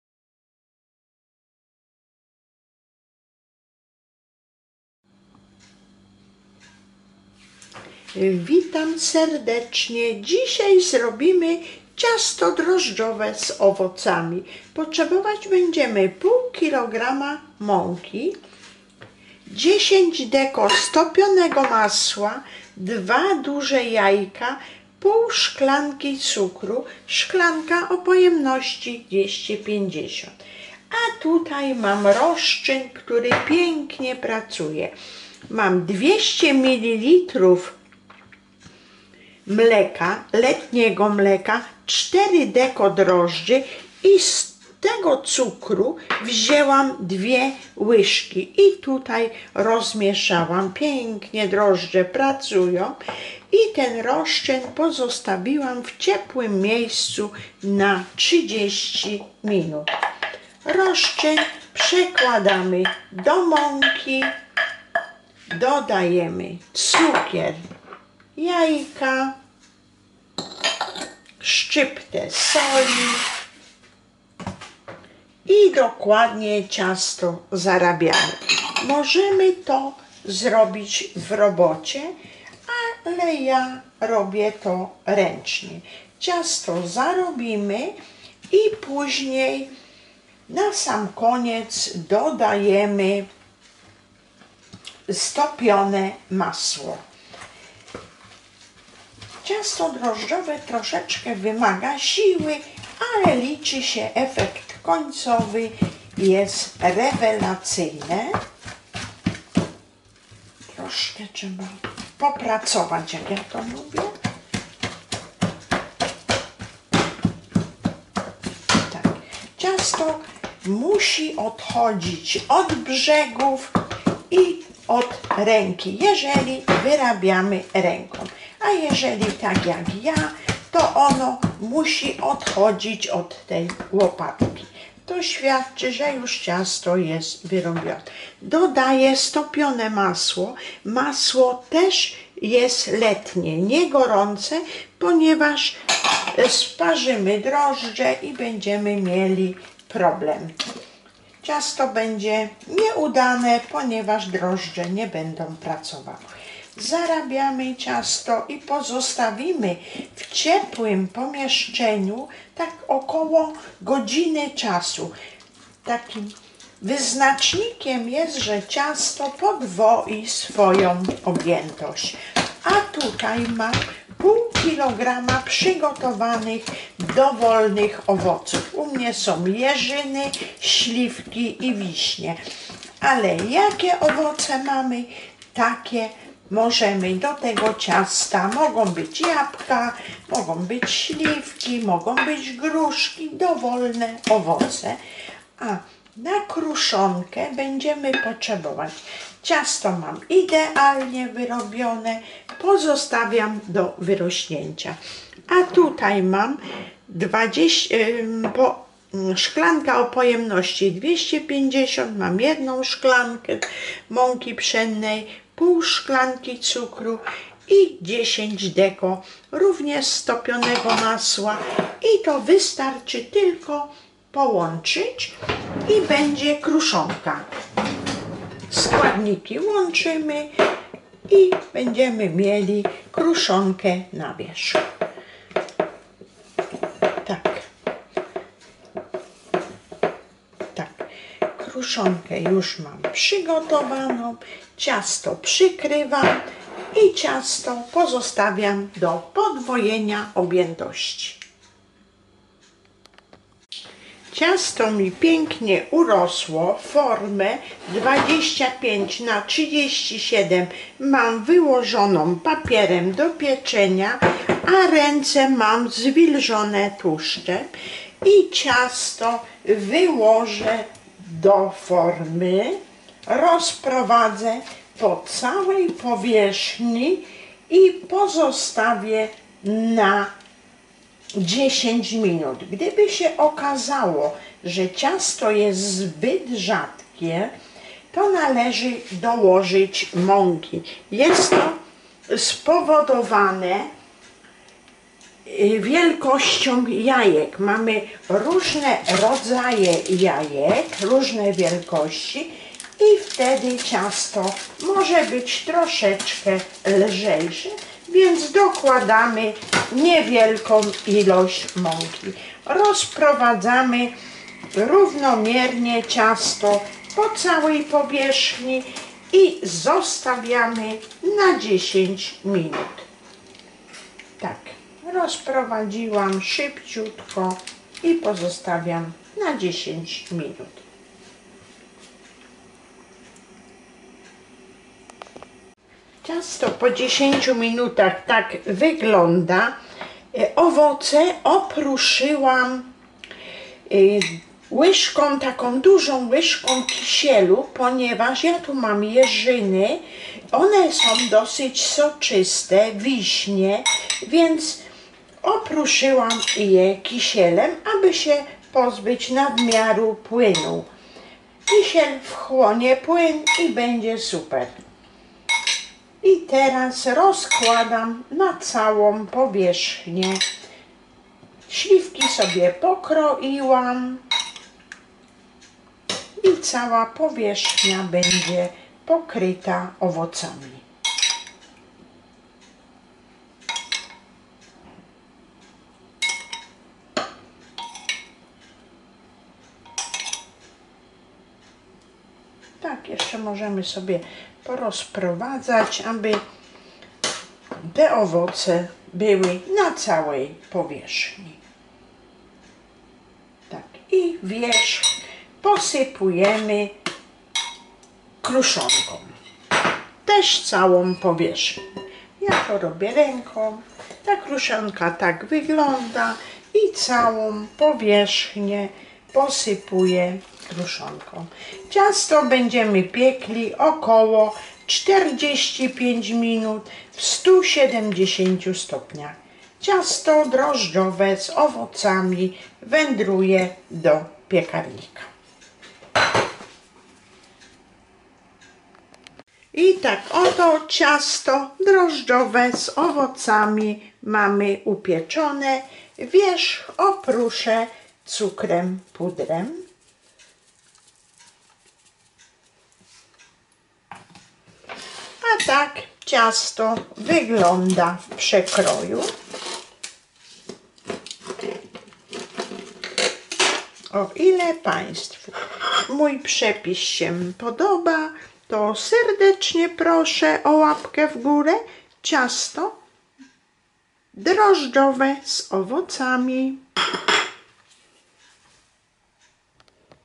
Moje ciasto urodzinowe: Ciasto drożdżowe z owocami
Z okazji moich urodzin przygotowałam pyszne ciasto drożdżowe z owocami.

Dziękuję !!

Składniki:

1/2 kg mąki pszennej
2 jajka
1/2 szklanki cukru
10 dkg masła
szczypta soli
200 ml mleka
4 dkg drożdży

Owoce:

60 dkg ulubionych owoców : wiśnie śliwki jeżyny
1 łyżka kisielu

Kruszonka:

1 szklanka mąki pszennej
1/2 szklanki cukru pudru
10 dkg masła

*szklanka o pojemności 250 ml

Sprawdzone, tradycyjne, domowe przepisy w nowoczesnym wydaniu.